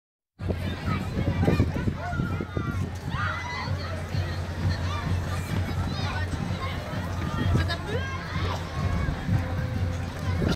Un avion largue une bombe sur une foule de gens au carnaval de Jargeau dans le Loiret.
Lors du carnaval de Jargeau, dans le Loiret, un avion de l'armée a lancé une bombe sur la foule trop nombreuse .

Beaucoup de blessés ? De morts ?

F A K E

FAKE MDR

carnaval, carnaval de Jargeau, Jargeau, carnaval 2019, bombe, attentat, avion lance une bombe, alerte attentat, foule attaquée,